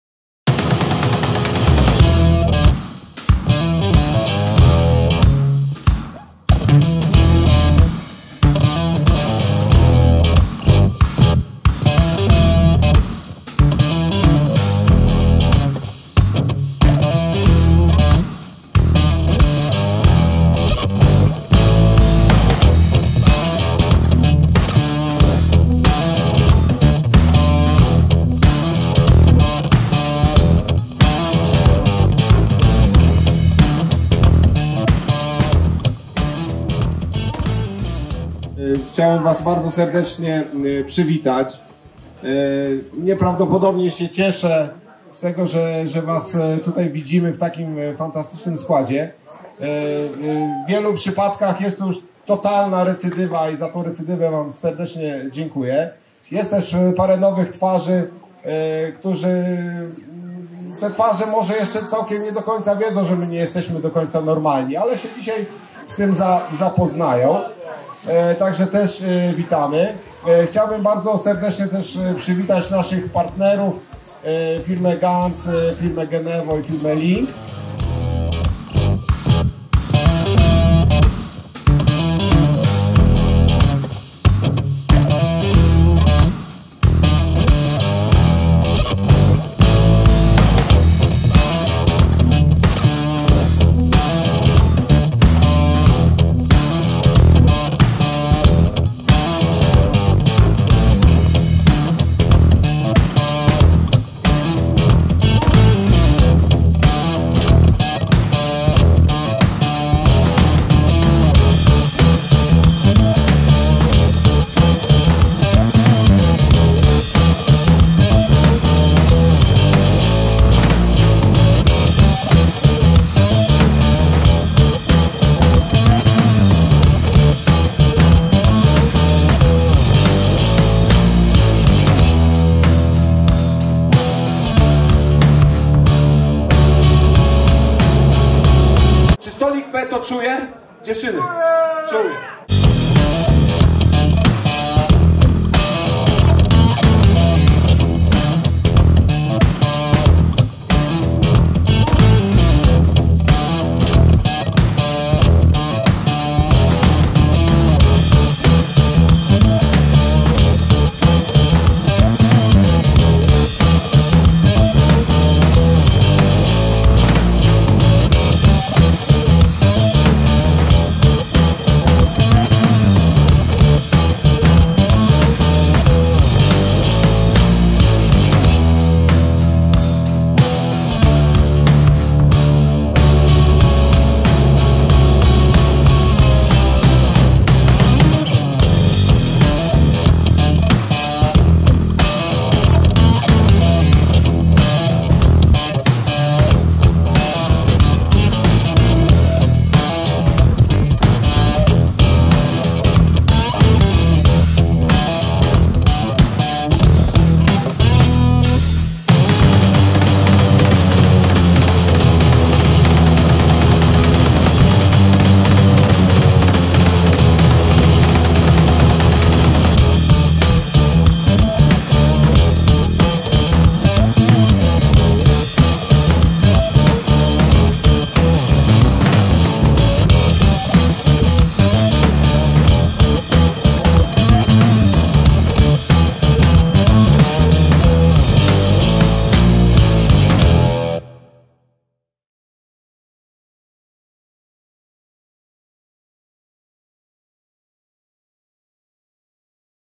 DMSI CUP 2020 - spotkanie branży ochrony i koncert zespołu Korzuh | SAFESTAR.PL
Poznaj SAFESTAR - jeden system, wiele możliwości dla branży ochrony.

Relacja wideo z pierwszego dnia DMSI CUP 2020. Nasza impreza odbyła się po raz drugi w Rynie w gościnnych progach Bocianiego Gniazda. Przywołajcie wspomnienia - tak było, tak było...prawdziwy ogień!!!

Oczywiście wielkie podziękowania dla naszych partnerów Linc Polska, Genevo, CBC Poland oraz dla naszego patrona medialnego A&S Polska.